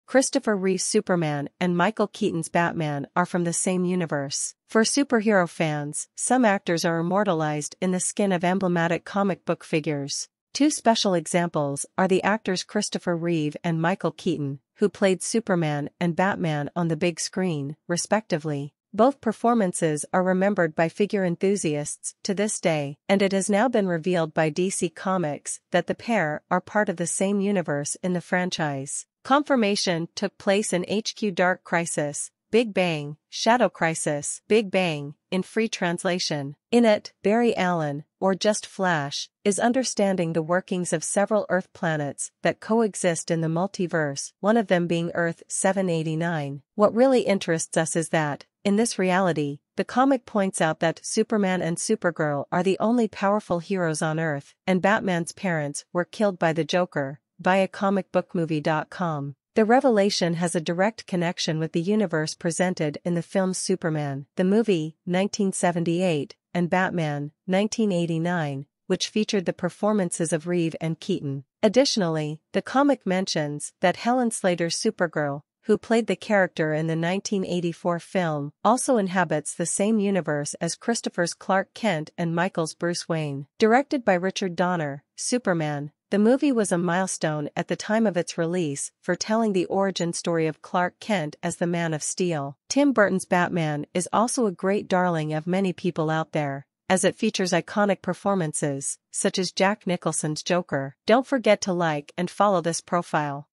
Christopher Reeve's Superman and Michael Keaton's Batman ARE FROM THE SAME UNIVERSE
For superhero fans, some actors are immortalized in the shoes of iconic comic book figures. Two special examples are the actors Christopher Reeve and Michael Keaton, who played Superman and Batman on the big screen, respectively. Both performances are remembered by figure enthusiasts to this day, and now it has been revealed by DC Comics that the duo is part of the same universe in the franchise.

The confirmation took place in the comic book Dark Crisis: Big Bang. In it, Barry Allen, or just Flash, is understanding the functioning of several Earth planets that coexist in the Multiverse, one of them being Earth-789. What really interests us is that, in this reality, the comic points out that “Superman and Supergirl are the only powerful heroes on Earth; and Batman's parents were killed by the Joker"

The revelation has a direct connection with the universe presented in the features Superman: The Movie (1978) and Batman (1989), which featured performances by Reeve and Keaton. Furthermore, the comic mentions that Helen Slater's Supergirl, who played the character in the 1984 film, also inhabits the same universe as Christopher's Clark Kent and Michael's Bruce Wayne.

Directed by Richard Donner, Superman: The Movie was a landmark at the time of its release as it told the origin story of Clark Kent as the Man of Steel. Tim Burton's Batman is also a great favorite of many people out there, as features iconic performances, such as Jack Nicholson's Joker.